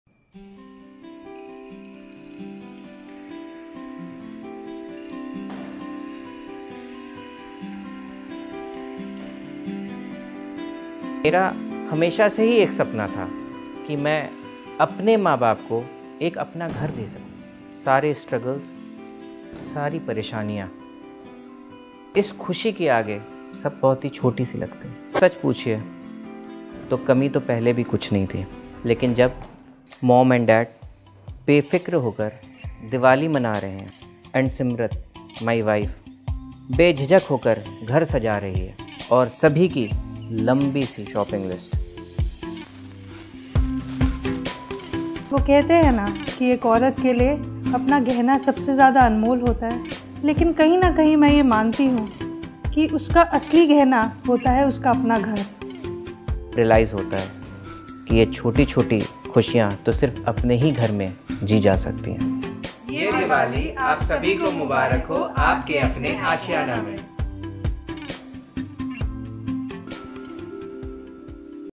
SmileWaliDiwali | New Home Celebrations
“I believe that the most valued possession in a woman's life isn’t her jewelry but, rather a house of her own”. Walk into Mrs. Simrat Saxena’s world as she takes us through a#n inexpressive journey of how her wishes and dreams become true as she captures it all in her own house, which indeed is her Ashiana.